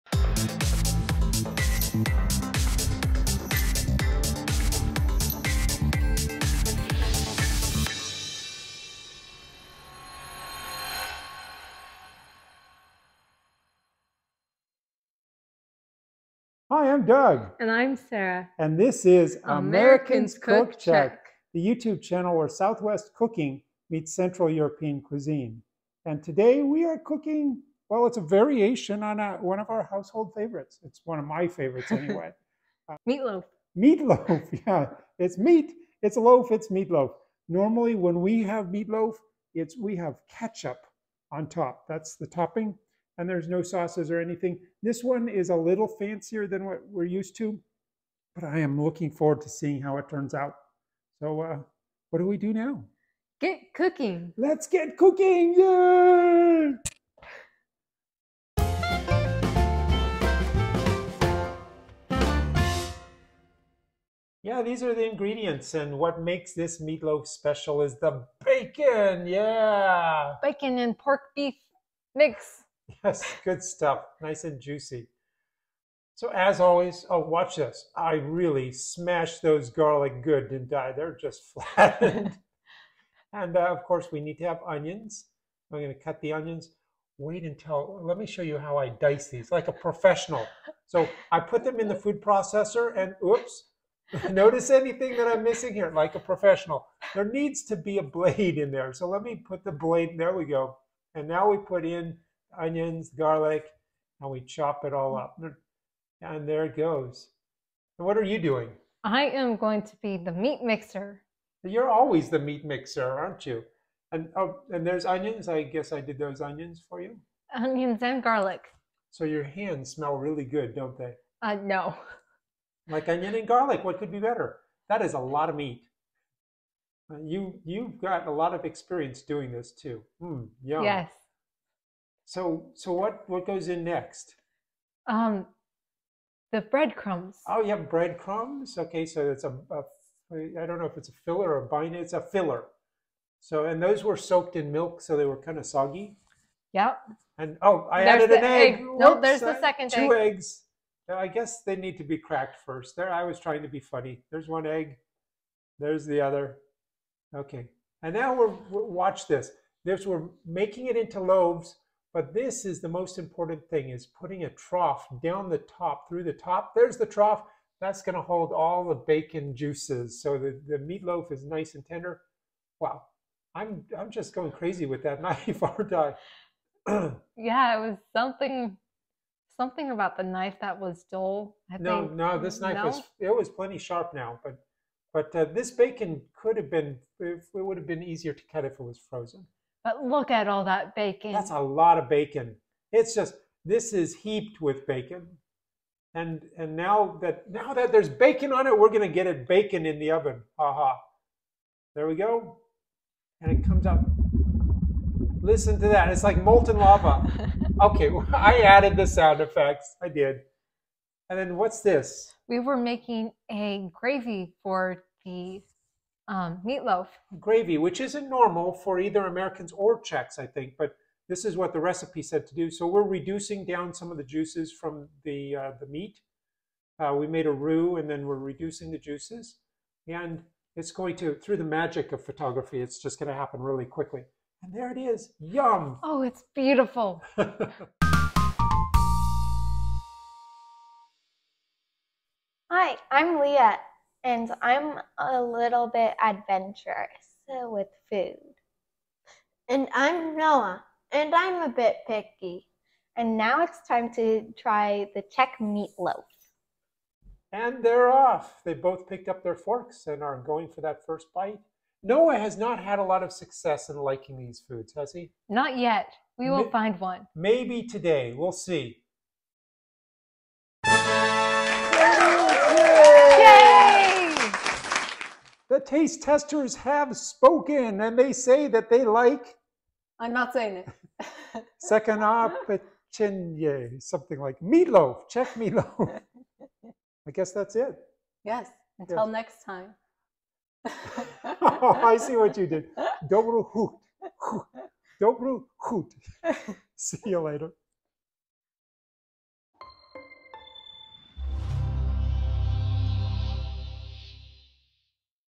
Sekaná Pečeně
Czech meatloaf with a little bit of something extra.  What will the taste testers think?  Do you like meatloaf or not?  Leave us a comment to let us know.
Just in case you are interested in the recipe, it can be found